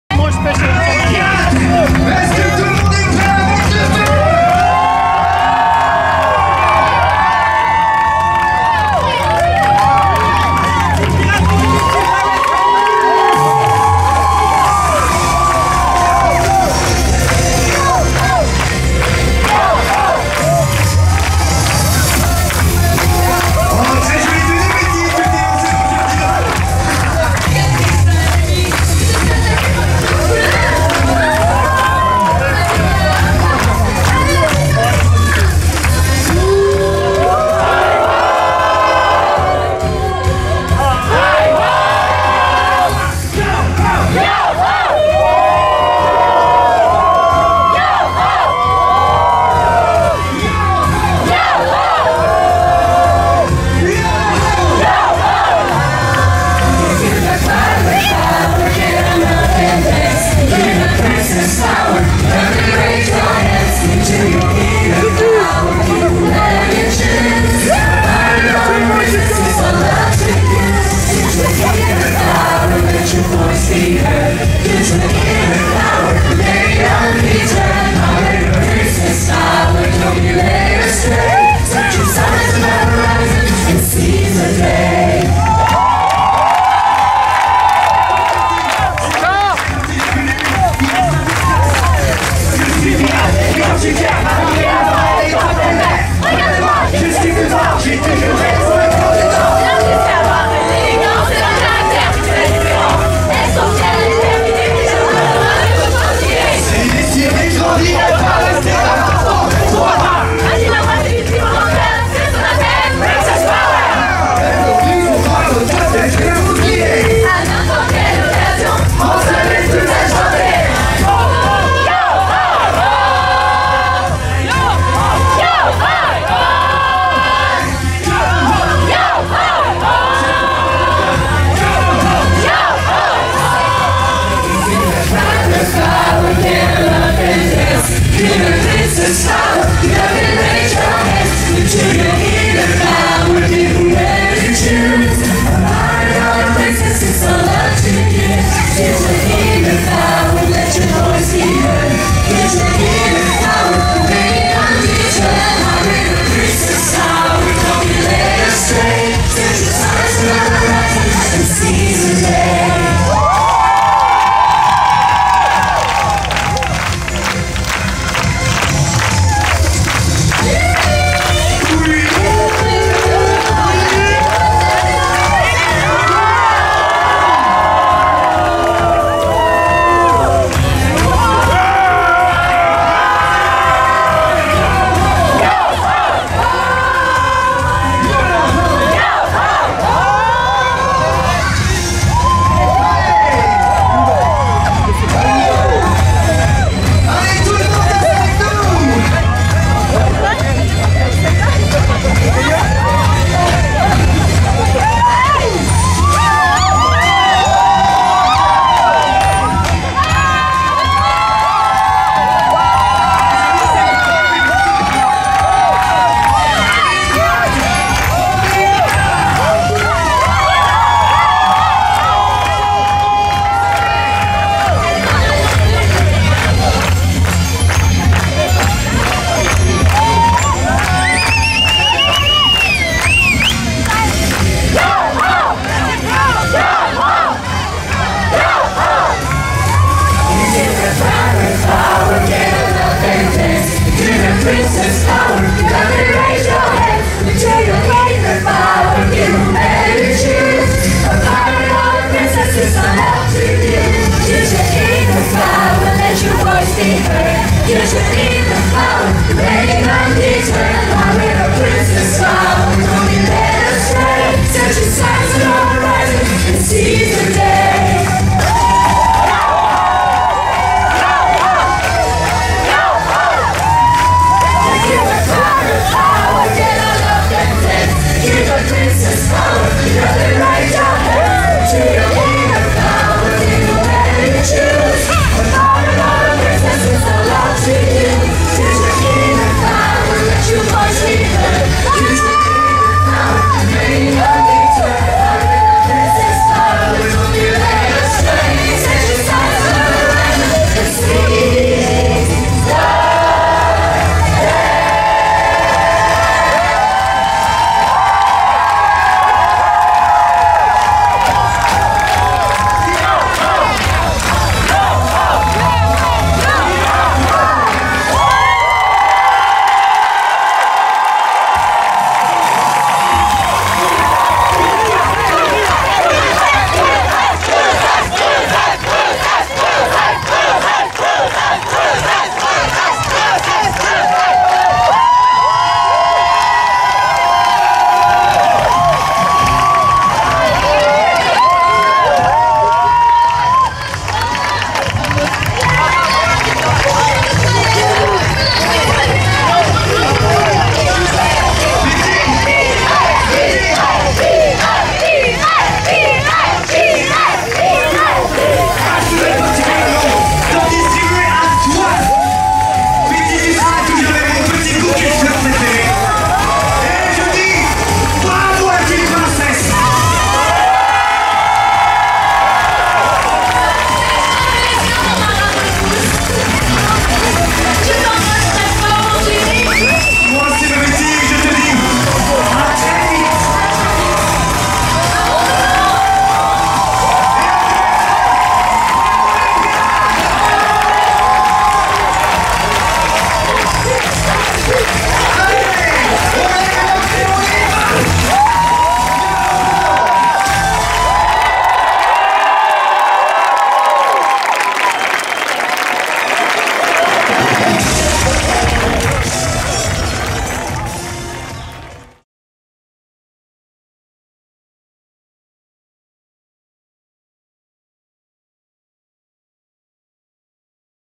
Dernière de Pirates & Princesse 2018
La dernière représentation du show Pirates & Princesses au central Plaza le 31 Mai 2018 à DisneyLand Paris

Un moment unique que l'on as le plaisir de vous montré.